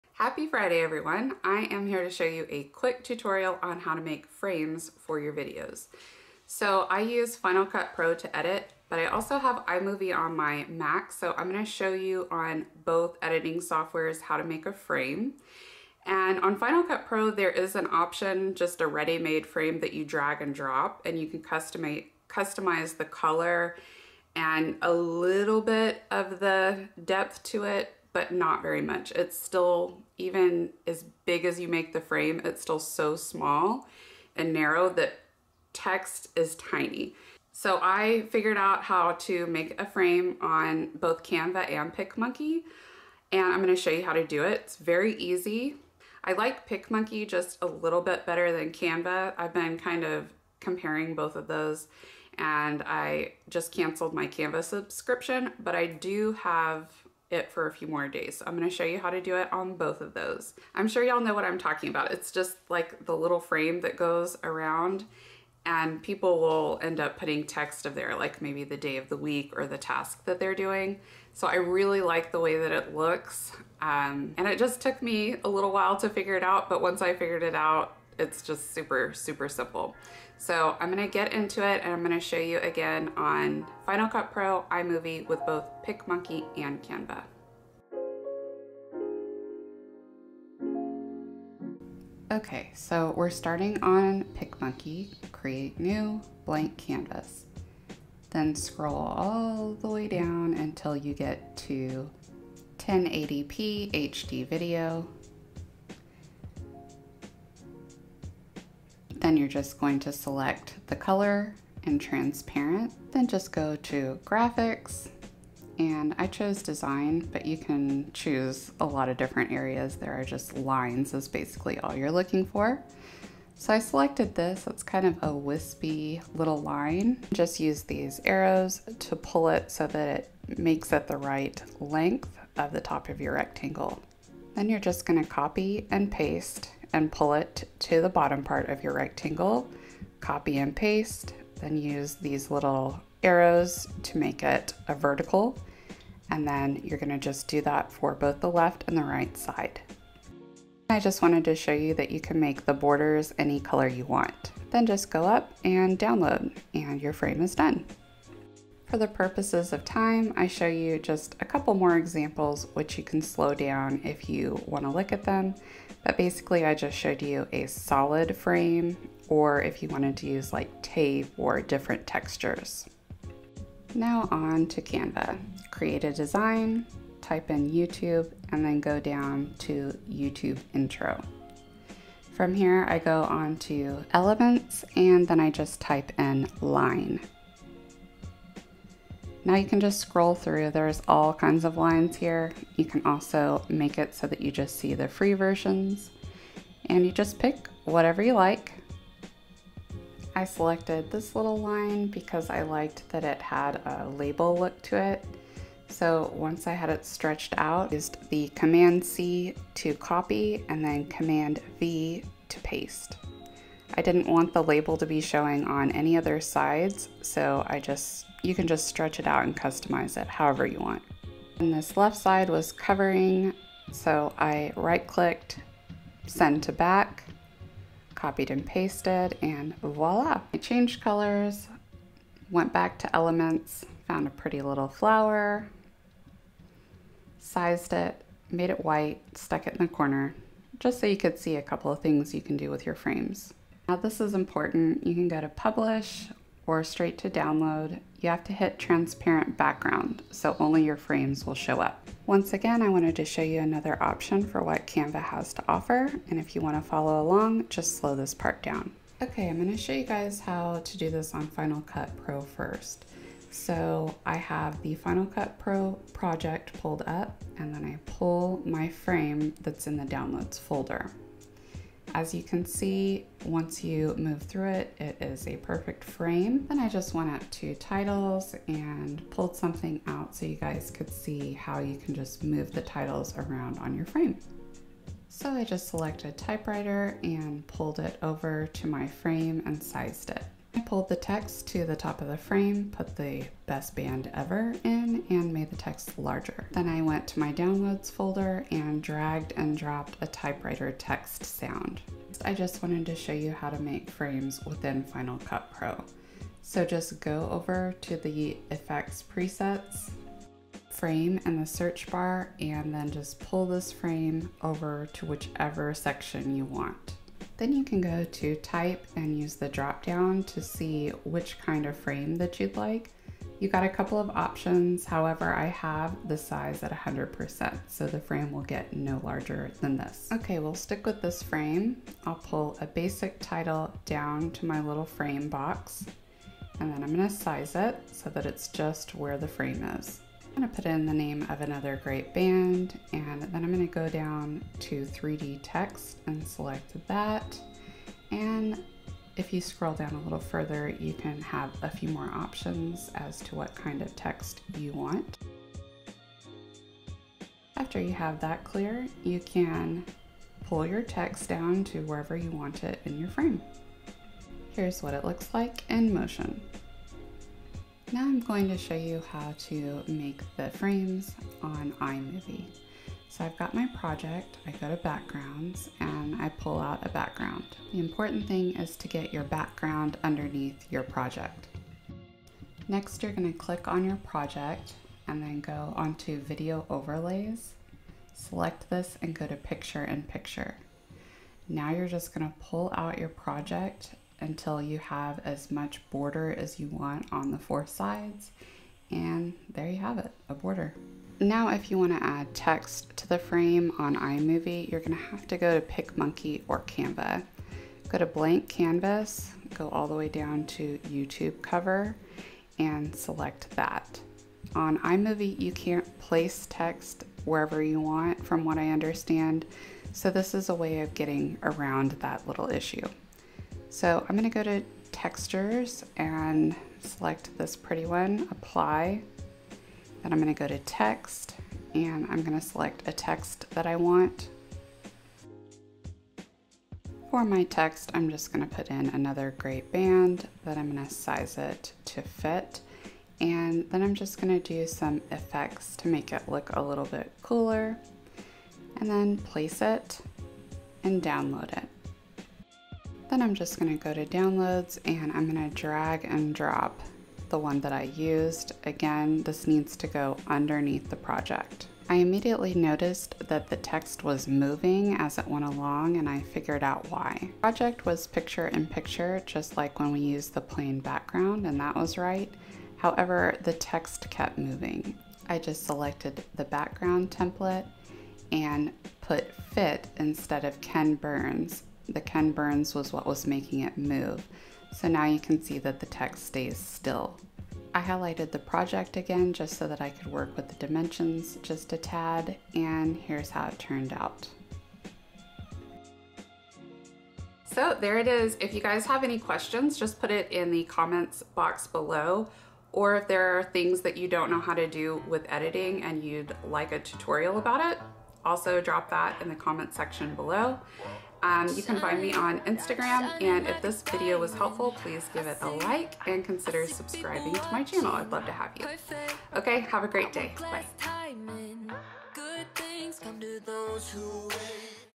How to Make Frames/Borders on FCP & iMovie || Use PicMonkey & Canva to Create Frames/Borders
Easy Editing Tips: Making Borders (or Frames) for both FCP & i-Movie. (I also troubleshoot text moving on iMovie.) Next, I demonstrate how to create borders for iMovie & FCP using PicMonkey & Canva. These tips are quick to learn and give your video added interest for the viewer. Hope you all are having a great day and I'll be back next Friday with more editing tips!

Let's get together on IG!